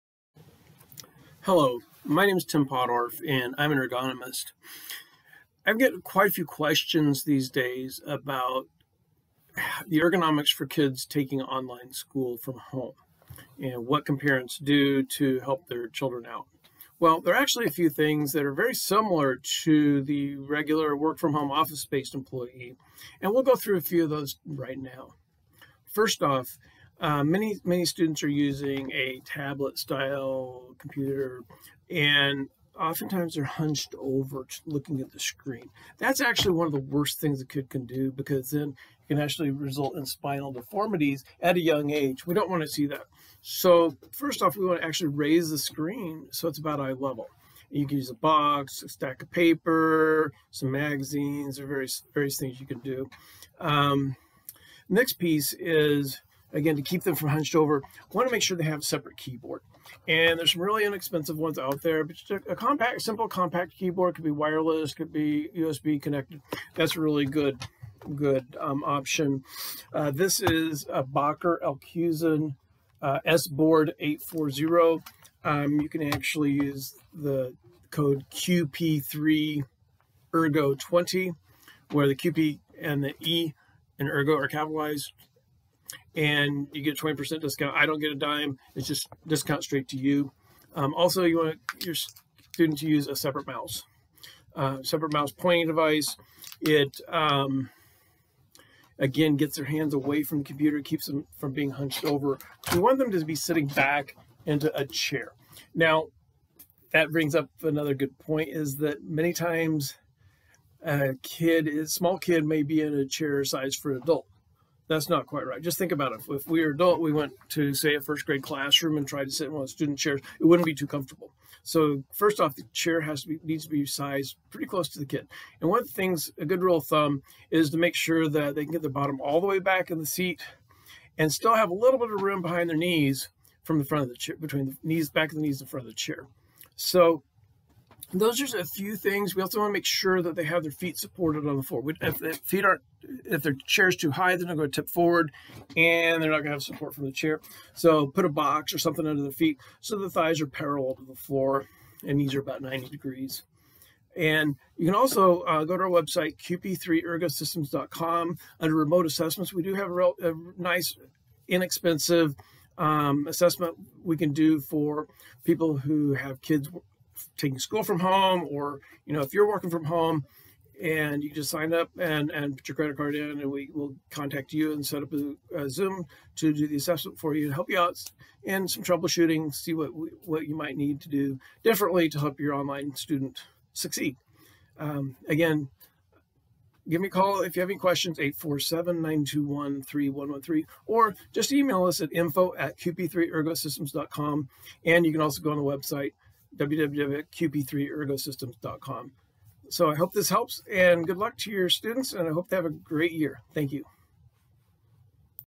QP3 Online School Ergo
With millions of school-age children taking classes online from home all or part of each week, their physical health may be at risk.  This brief video provides parents and students with guidance on good ergonomics practices for online school.